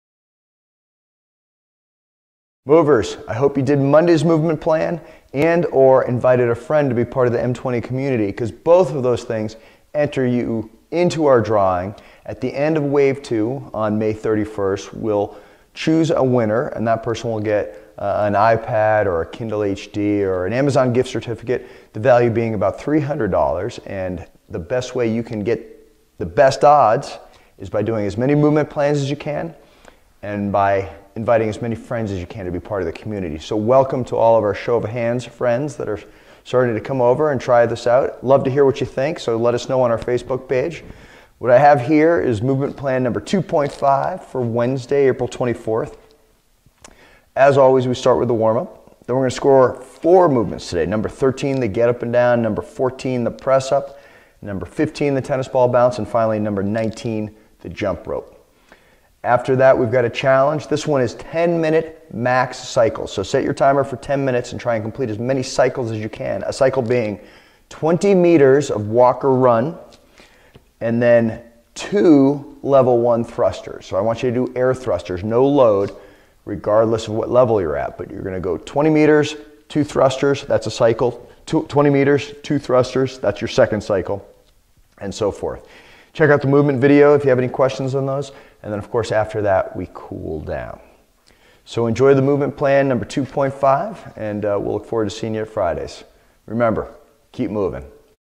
MP 2.5 - Wednesday, April 24 - M20 Fitness
a. Warm Up
b. Score - Get Up and Down, Press Up, Tennis Ball Bounce, Jump Rope
c. Challenge - 10 Minutes MAX Cycles - A cycle is 20 Meters Walk/Run, 2 Level 1 Thrusters
d. Cool Down